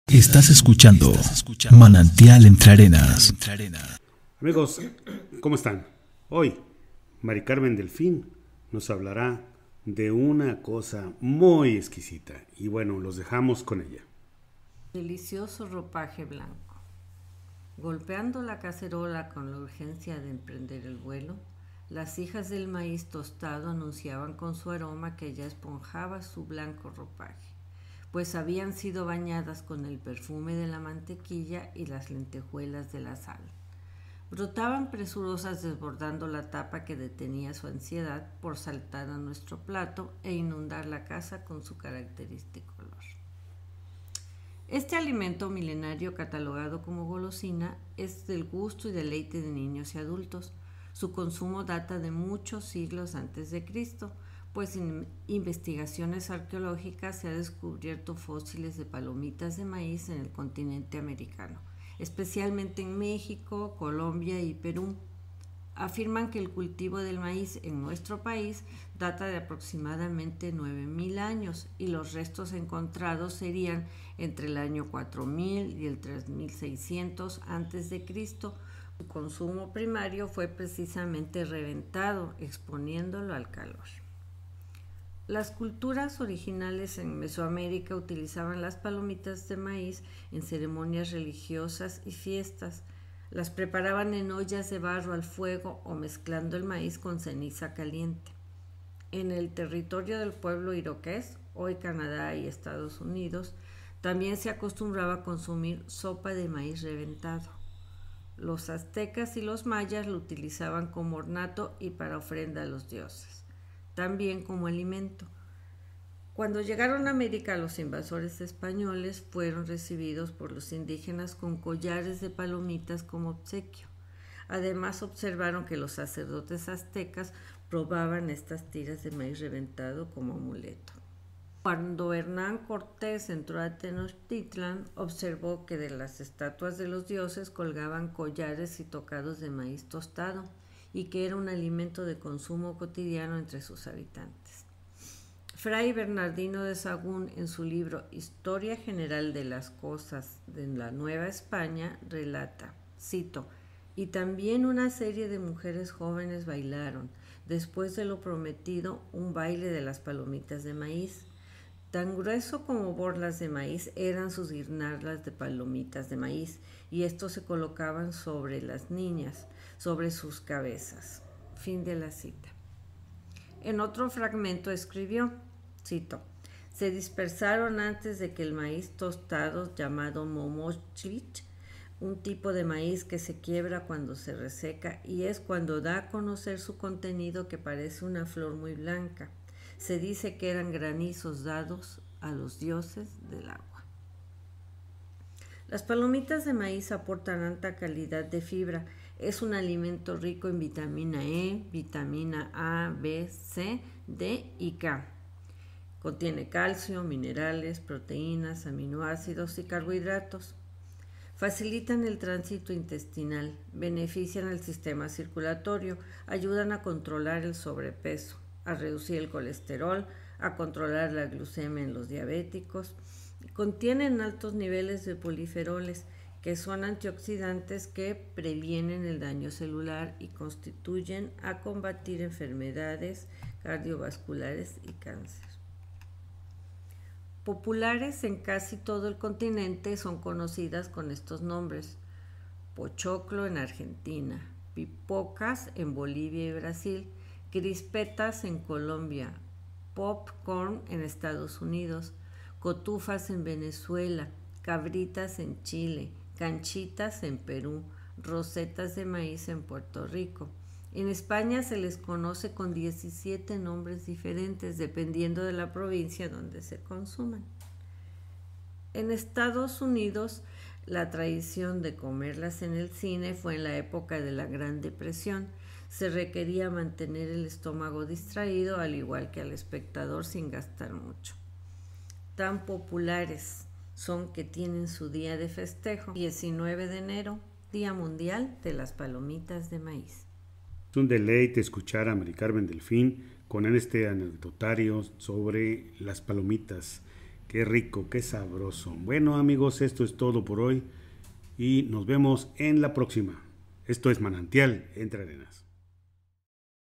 Delicioso ropaje blanco
Desde la época prehispánica el maíz en todas sus formas y variedades, una de ellas son "las palomitas" aquí su historia.